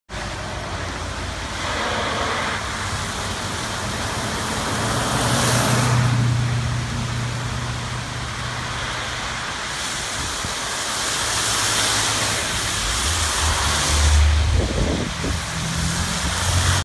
Storm
Heavy rains that hit Sonoma County closed certain parts of Petaluma Boulevard North